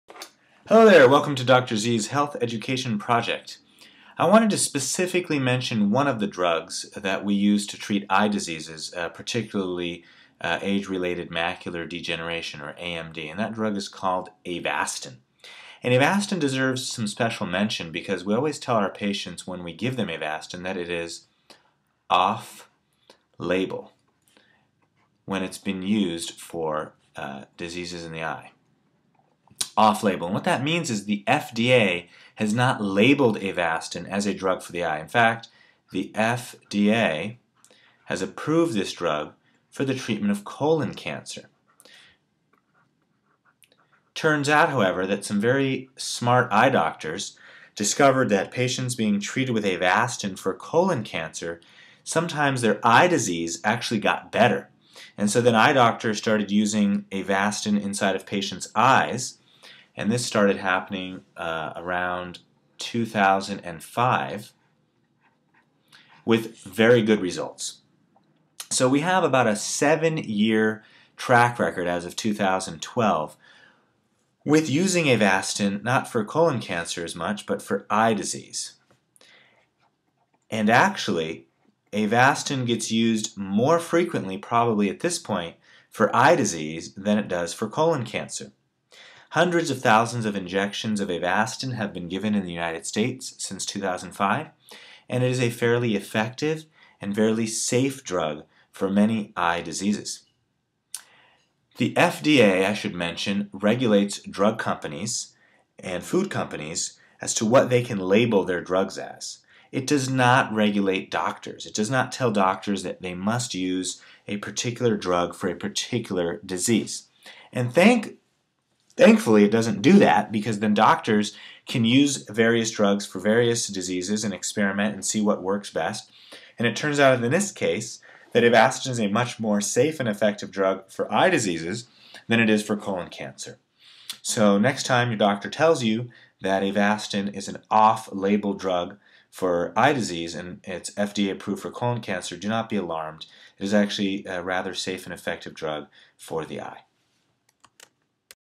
Avastin for eye disease
Welcome to Dr. Z's health academy.  In this video, I talk about Avastin (Bevacizumab) for eye disease.  This video does not substitute for consultation with your physician.